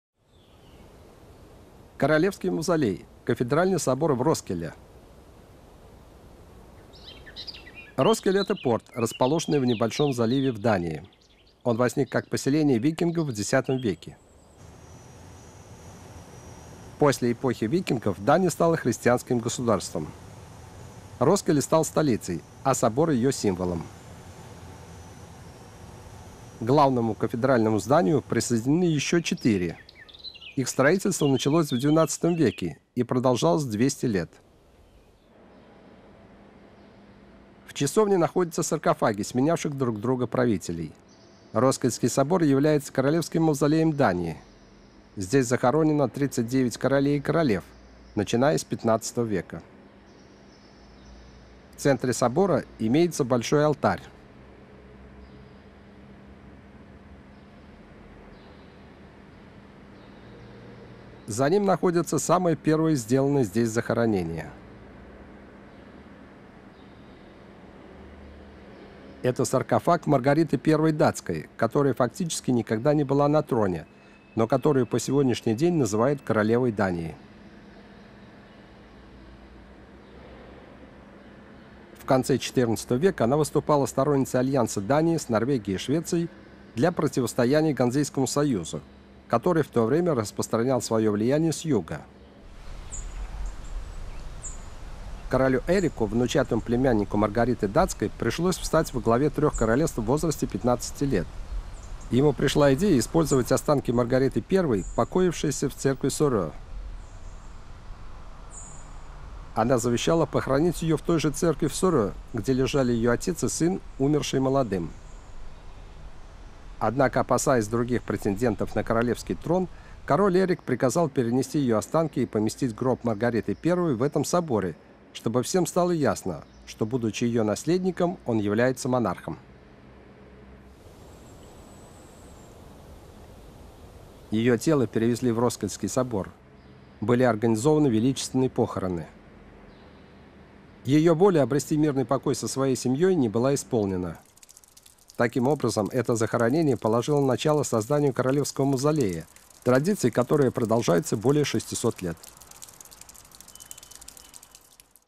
Кафедральный собор в городе Роскилле (UNESCO/NHK)
Построенный в XII-XIII вв., этот возведенный из кирпича первый готический кафедральный собор в Скандинавии послужил примером для распространения этого стиля по всей Северной Европе. С XV в. собор стал усыпальницей датского королевского семейства. В конце XIX в. к зданию собора были пристроены портики и боковые часовни. Все эти строения иллюстрируют ход развития европейской религиозной архитектуры.